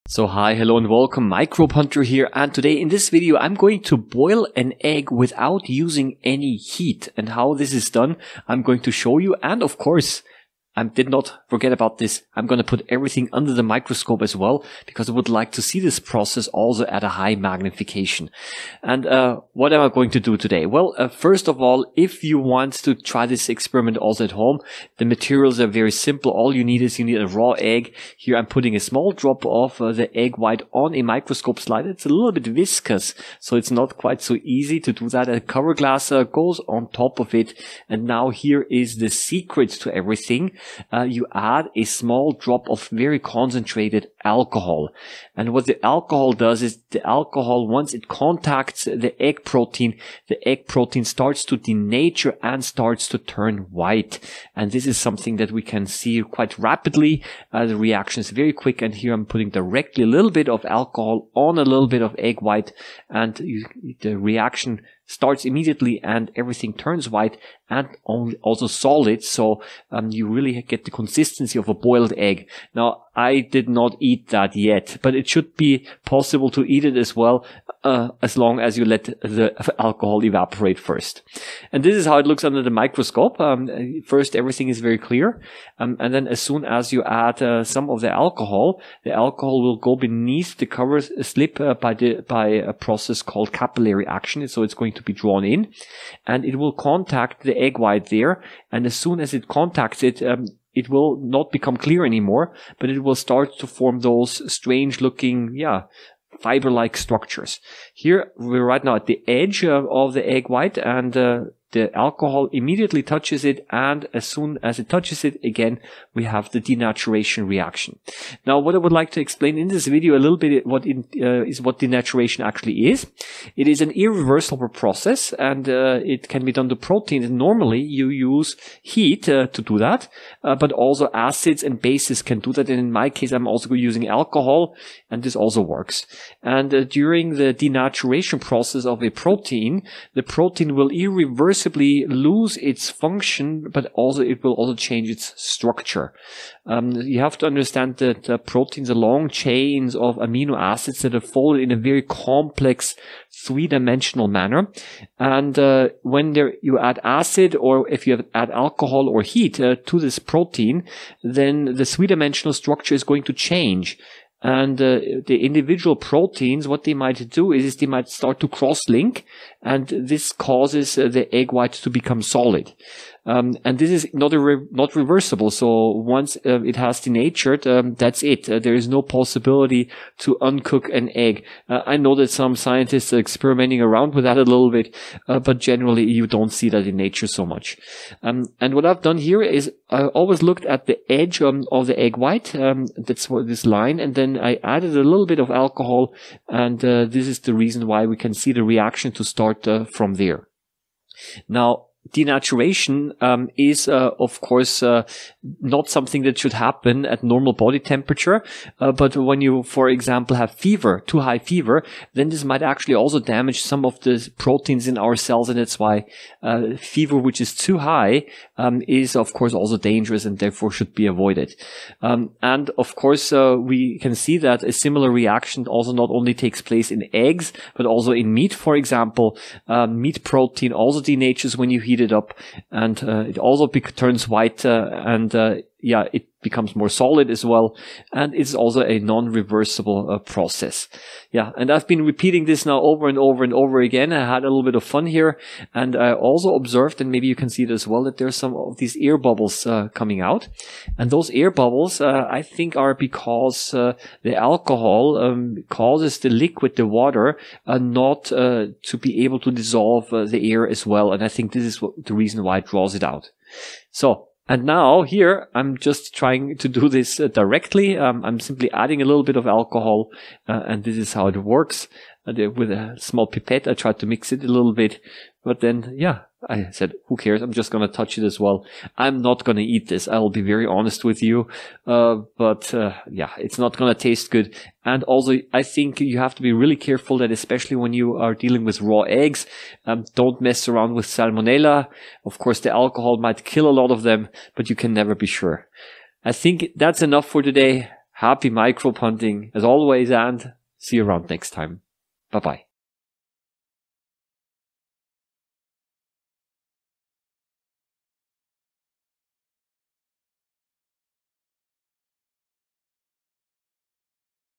How to Cook an Egg Without Heat
It is possible to denature egg protein without heating it. This denaturation process can be seen under the microscope, and this is what I show you here.

Stellar 1-T (Achromat)
Stellar 1 Pro-T (Plan)
SW150 (introductory microscope)
Slides, Cover Glasses